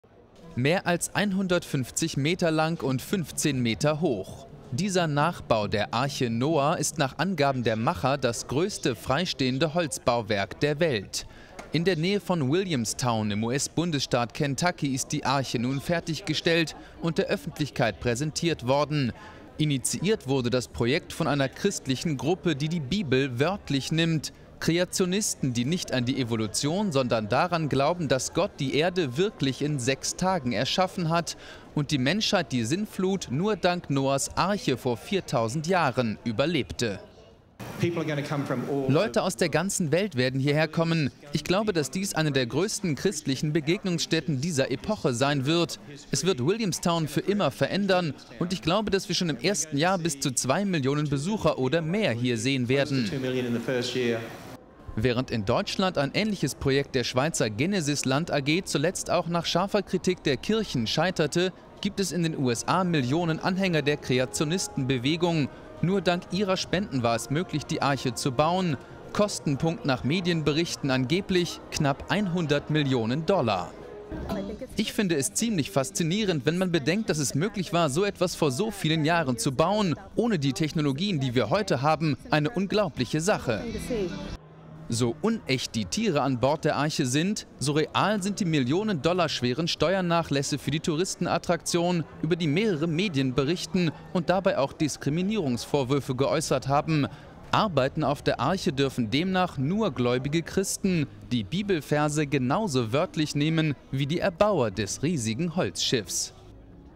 Riesig und umstritten: "Kreationisten" bauen Arche Noah nach
Sie nennen sich "Answers in Genesis" und nehmen die Bibel wörtlich: Im US-Bundesstaat Kentucky hat eine Gruppe sogenannter Kreationisten die Arche Noah nachgebaut. Das Projekt wird kontrovers diskutiert.